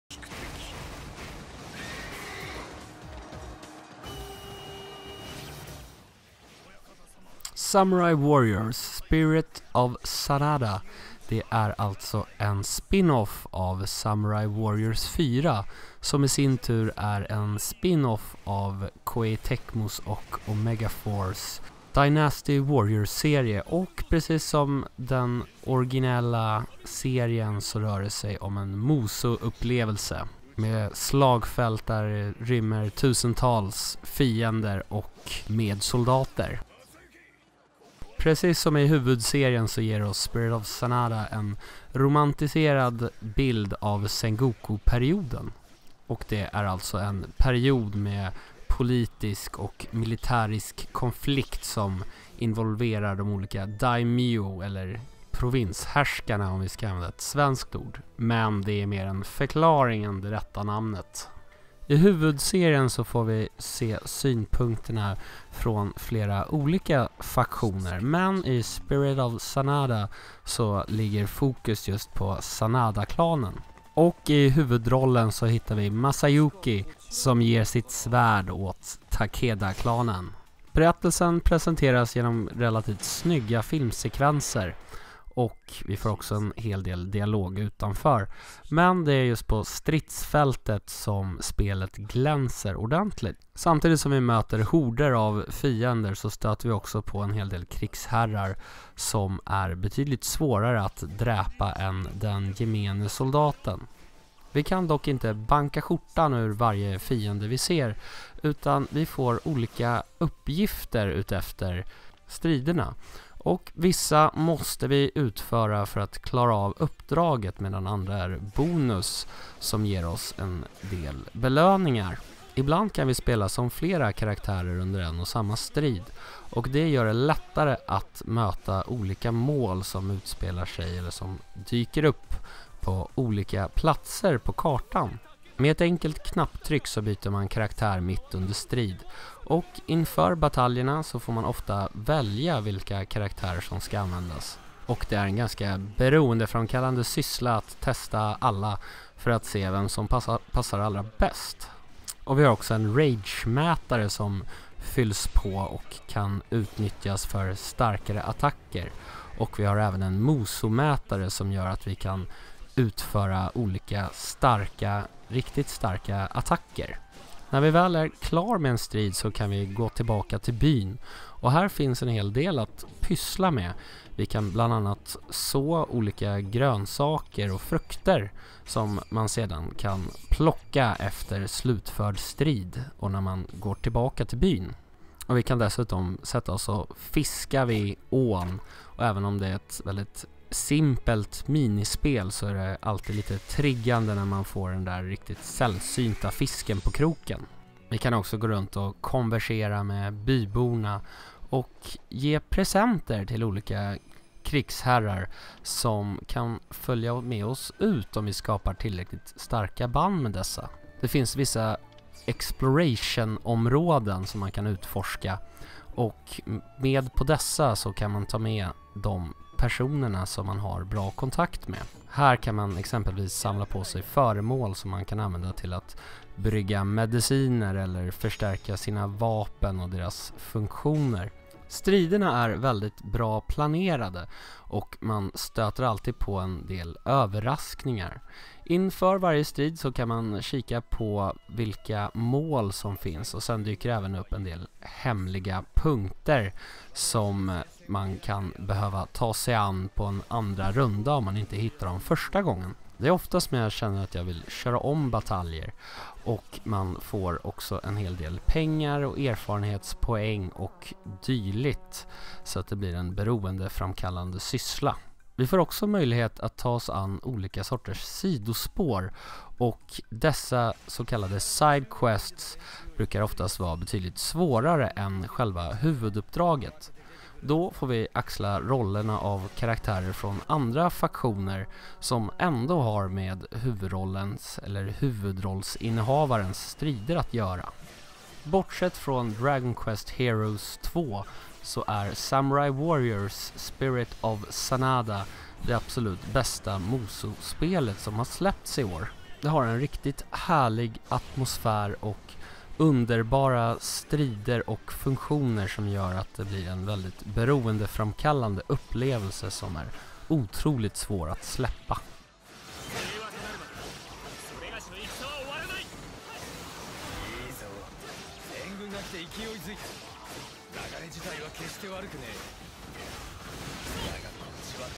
Recension: Samurai Warriors - Spirit of Sanada (PS4, PC)
Koei Tecmos Omega Force-studio ger oss ett av årets hittills bästa musou-upplevelser med actionrollspelet Samurai Warriors: Spirit of Sanada.